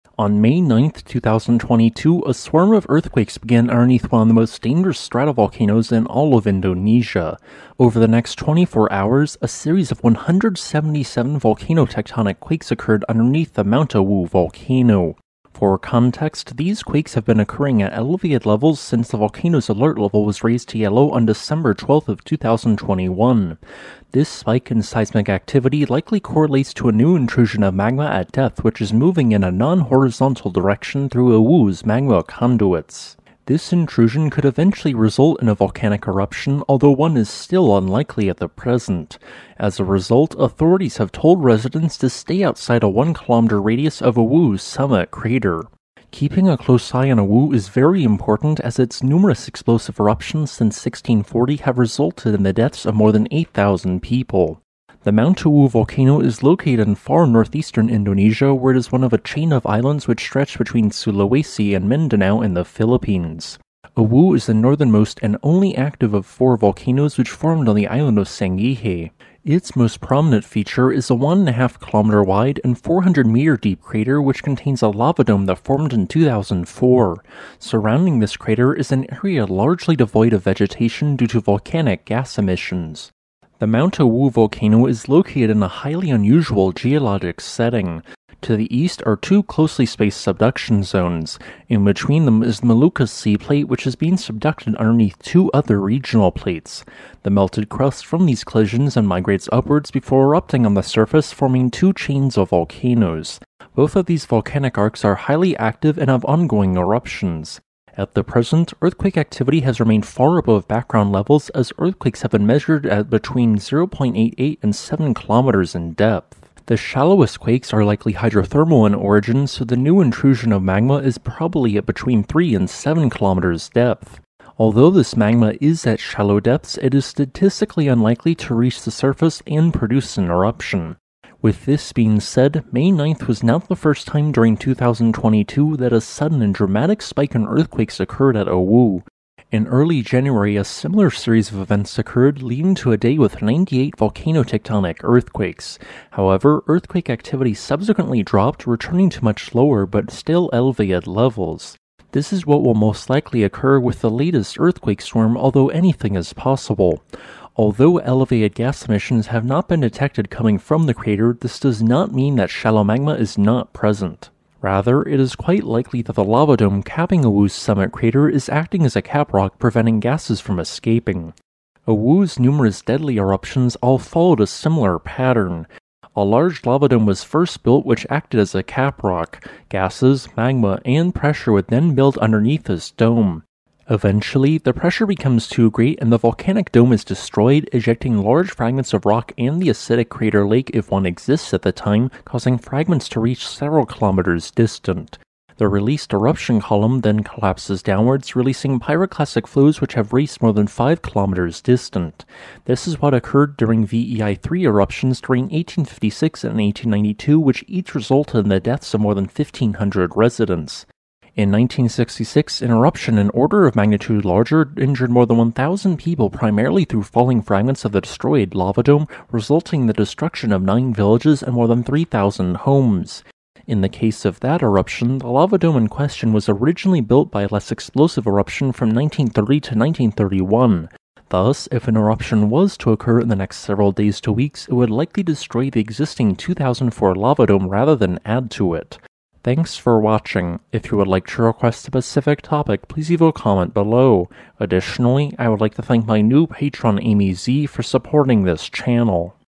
Mount Awu Volcano Update; Magma is on the Move, Earthquake Swarm
The dangerous Mount Awu volcano is showing signs of unrest north of the island of Sulawesi, as more than 170 volcanic earthquakes have occurred underneath its edifice. These quakes are almost all magmatic in origin, indicating a new intrusion of magma is pushing towards the surface. Mount Awu has not erupted since 2004 and has the potential to be quite dangerous due to the natural caprock the lava dome built in 2004 in its central crater. If an eruption were to occur, the lava dome would be destroyed, creating a similar eruption to what occurred at this volcano in 1812, 1856, 1892, or 1966.

MAGMA Indonesia (click here for updates on this volcano)

Thumbnail Photo Credit: ⒸGoogle Earth, Ⓒ2022 Maxar Technologies

0:00 Awu Earthquake Swarm
0:55 Background Information
1:24 Geologic Setting
1:48 Earthquake Interpretation
2:52 Eruption Cycle
3:23 Historical Eruptions